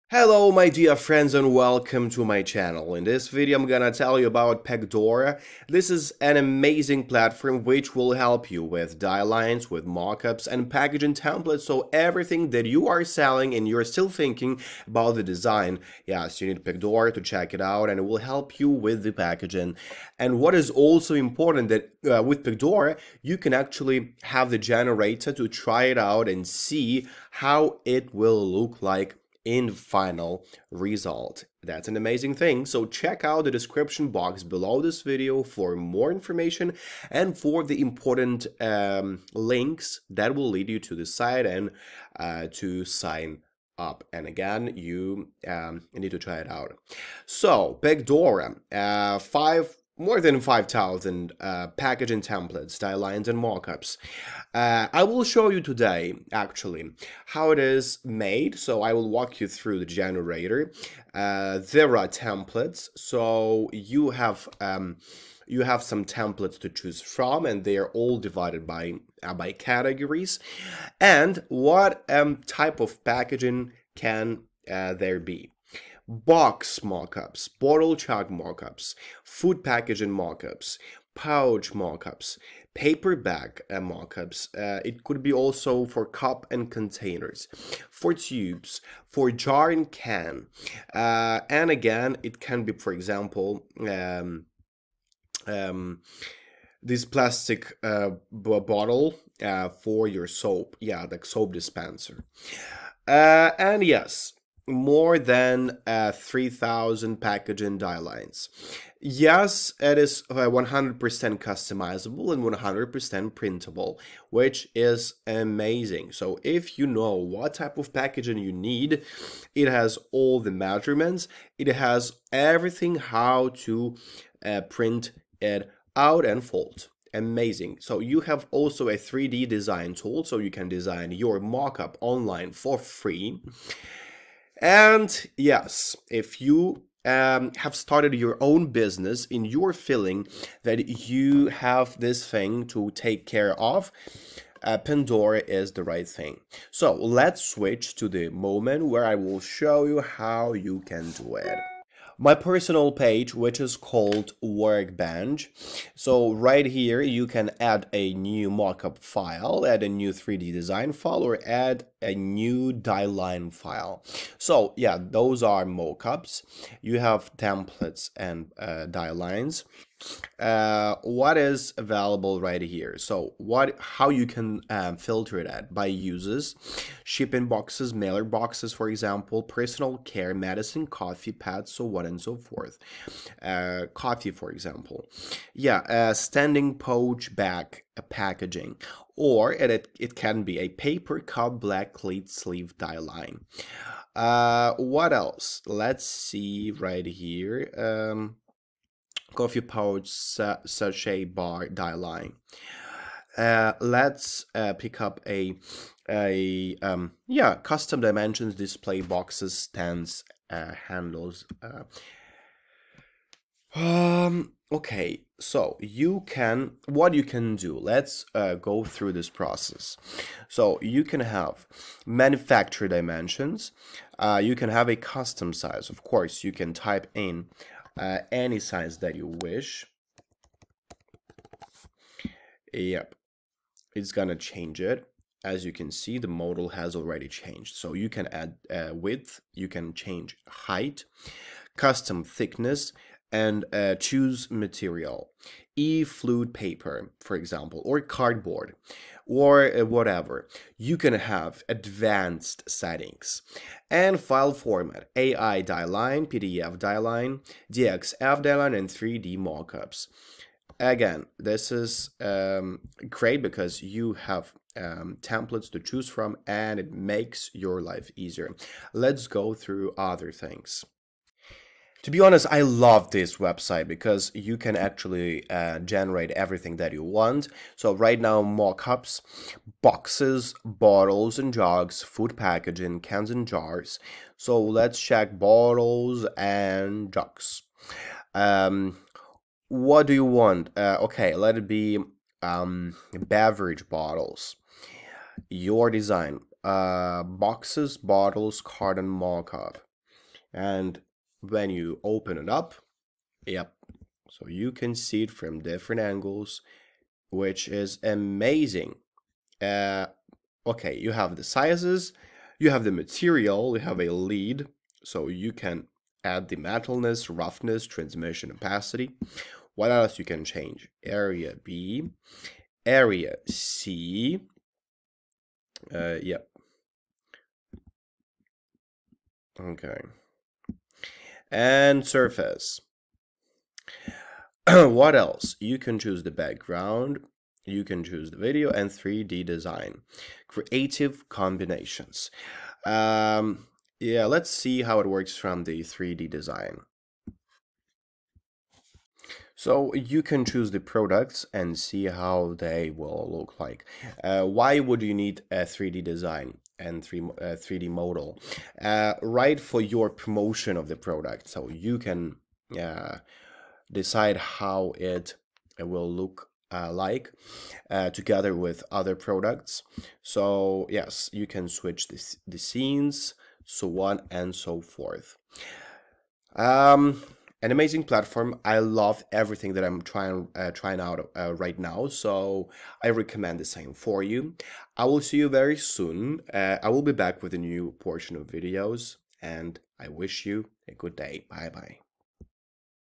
Pacdora's AI-Powered Packaging: A Design Revolution! 🔥
for the latest in AI packaging design!
Discover Pacdora: The AI-Powered Packaging Design Revolution! 🌟 | Explore How Pacdora's Cutting-Edge Technology is Transforming the Industry! 📦✨

Join us as we delve into Pacdora's innovative AI platform that's changing the game in packaging design. Learn how businesses are saving time and costs with stunning, efficient designs generated by artificial intelligence. Hear from industry experts on the impact of this groundbreaking technology and what it means for the future of packaging. Don't miss out on this exciting journey into the future of design!